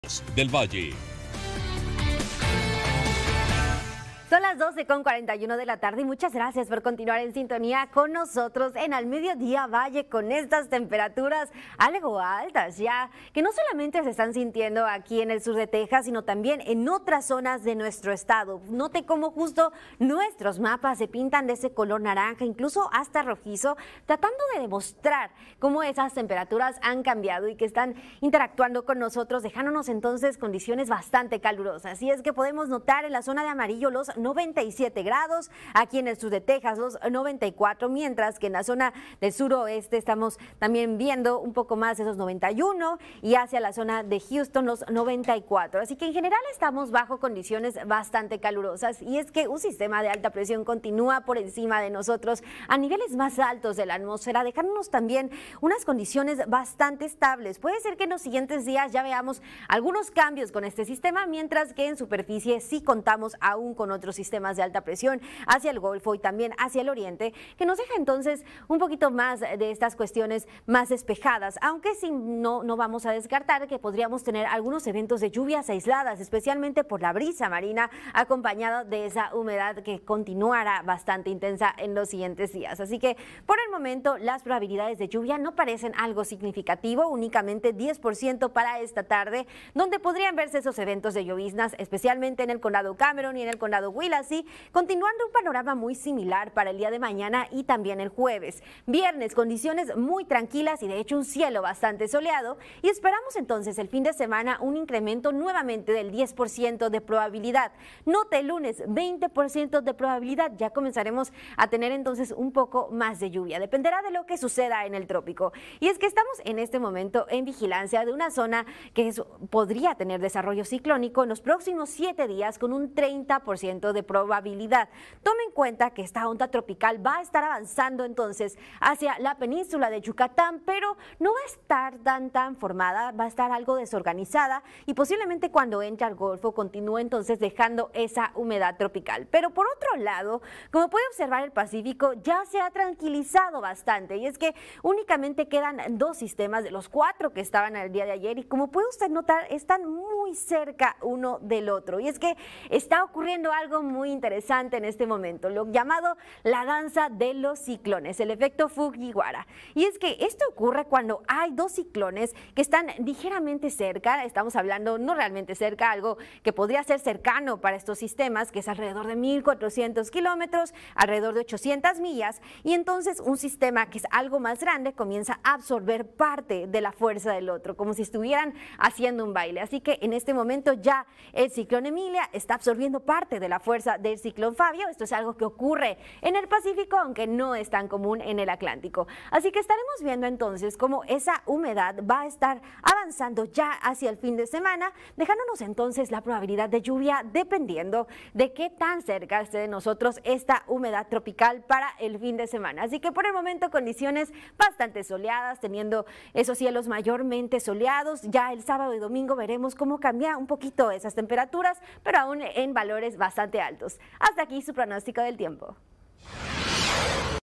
El Pronóstico del Tiempo con Marlenne Trujillo: 06 de Agosto, 2024
Pronóstico en el Valle RioGrandeValley 🌴

Condiciones calurosas, cielos despejados por una alta presión a niveles altos. Seguimientos de los ciclones y el efecto